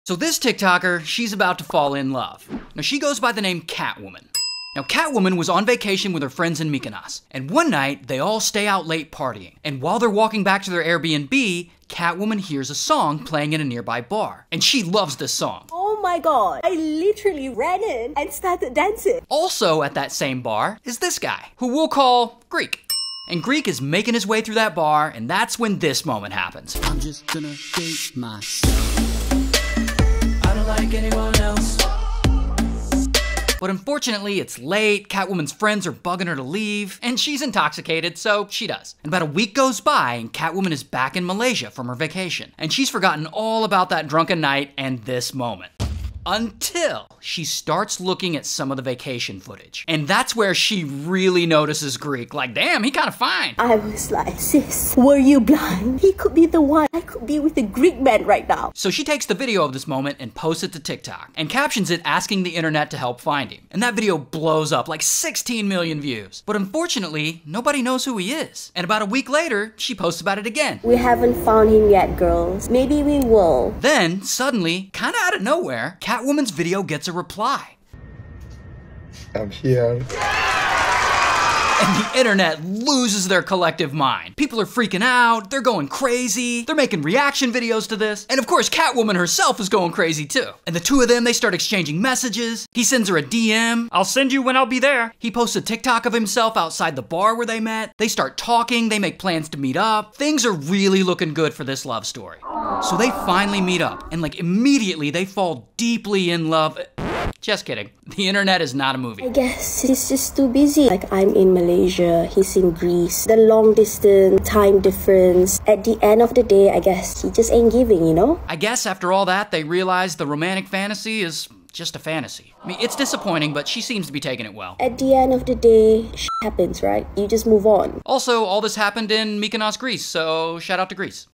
She finally did it
A TikToker from Malaysia meets the man of her dreams in Mykonos Greece. She asks TikTok to help her find him so they can link up.